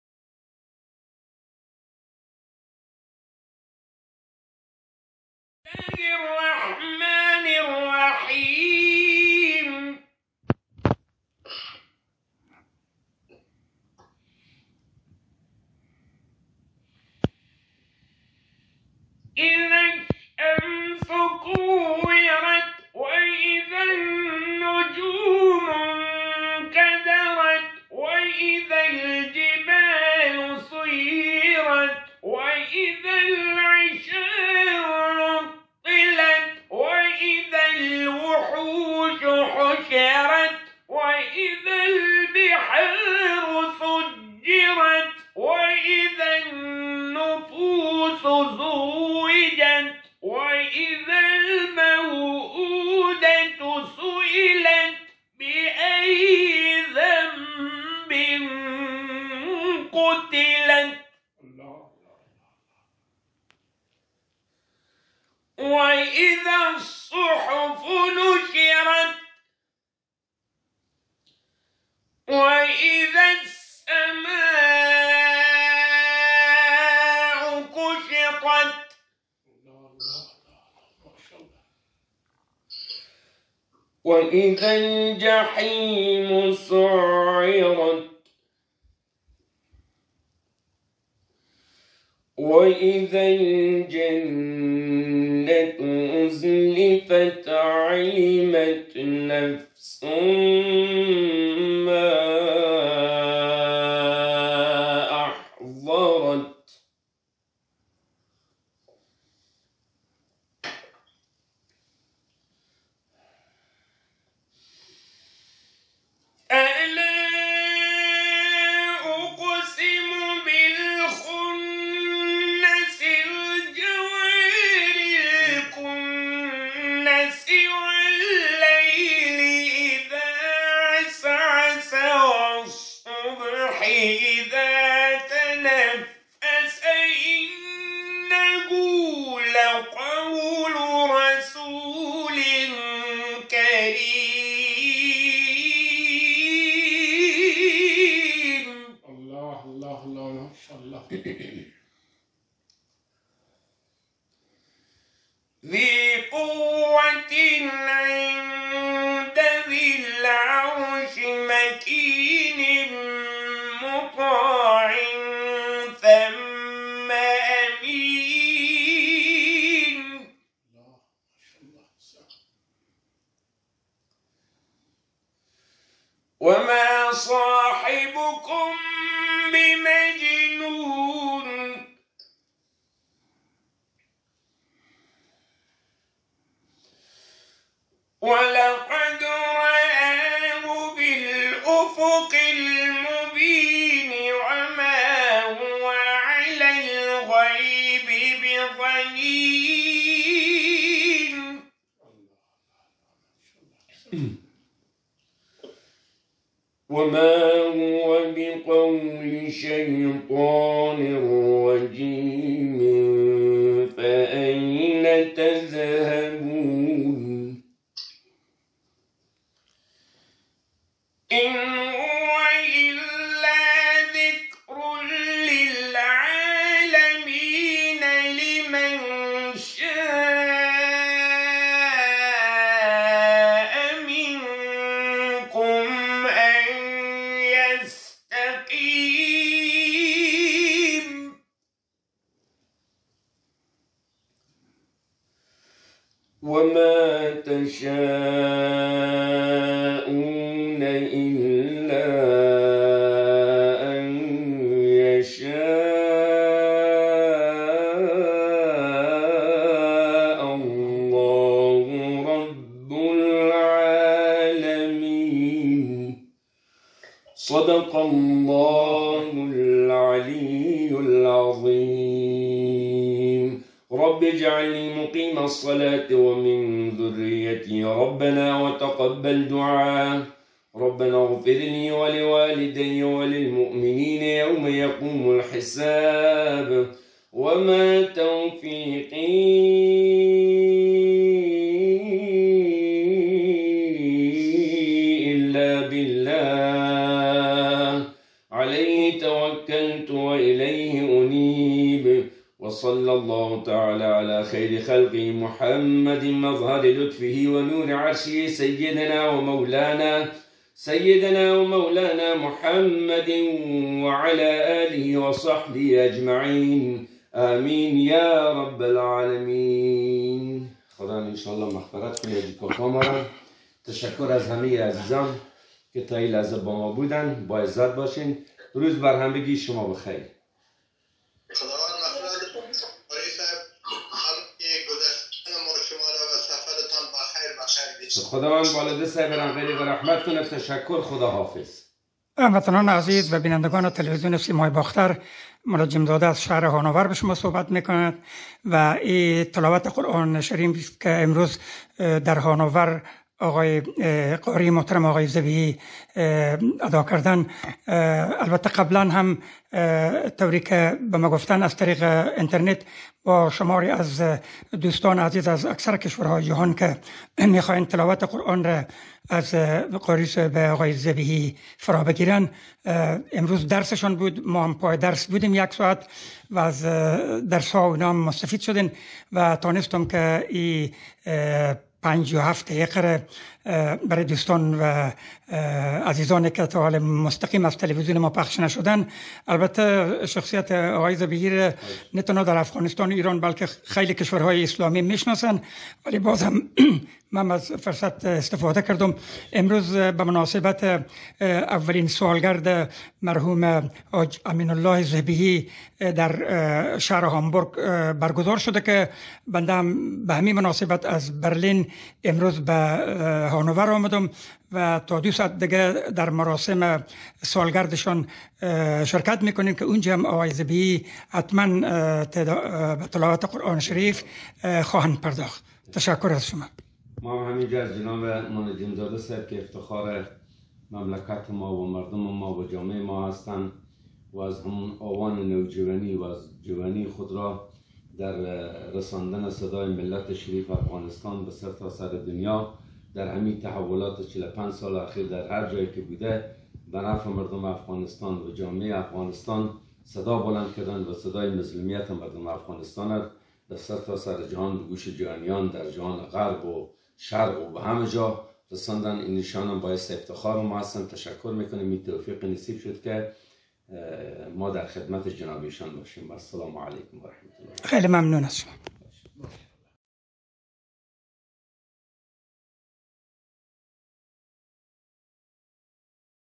TV Bachtar berlin تلویزیون سیمای باختر برلین تلاوت قران شریف توسط محترم قاری ذیبحی
تلاوت قران شریف توسط محترم  قاری ذیبحی بماسبت سالگرد  درگذشت زنده یاد حاج امین الله ذبیحی  در هانور